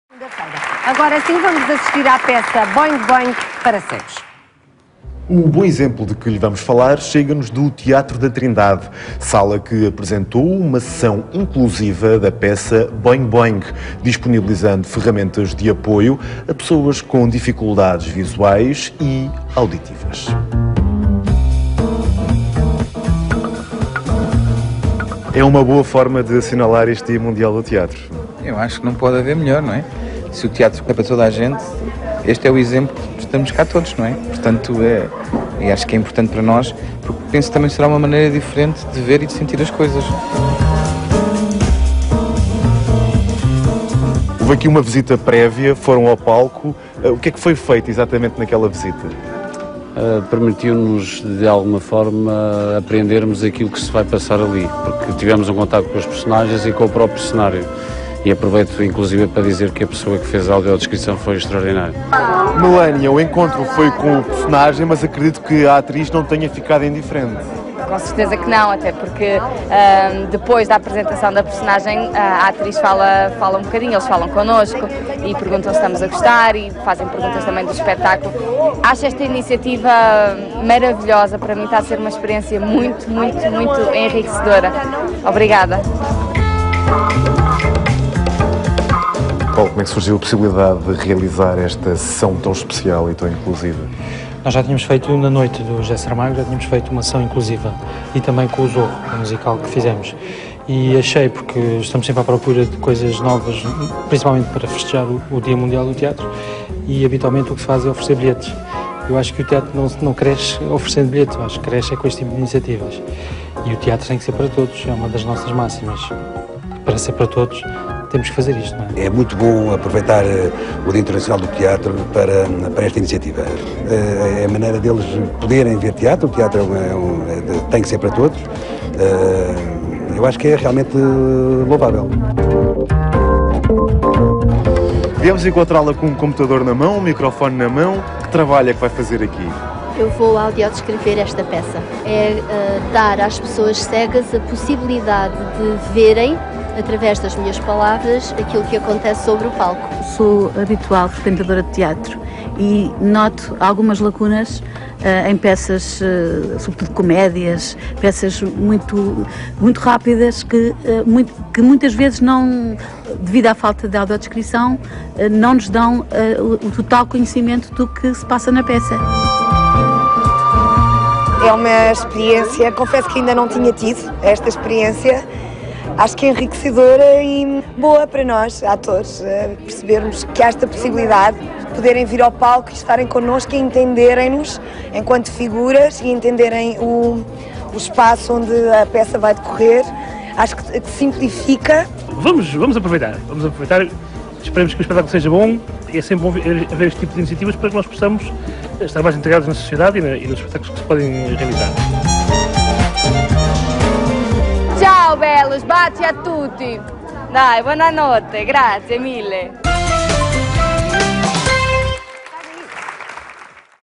"Boeing Boeing" - Sessão com audiodescrição e linguagem gestual
Apesar de todas as salas esgotadas que tivemos, que é sempre muito bom, este foi claramente o dia mais marcante da carreira do BOEING-BOEING no Teatro da Trindade. Porque o teatro TEM de ser acessível a TODOS!

Obrigado à RTP por ter percebido e ajudado a divulgar algo que é tão simples e ao mesmo tempo tão importante para a vida destas pessoas...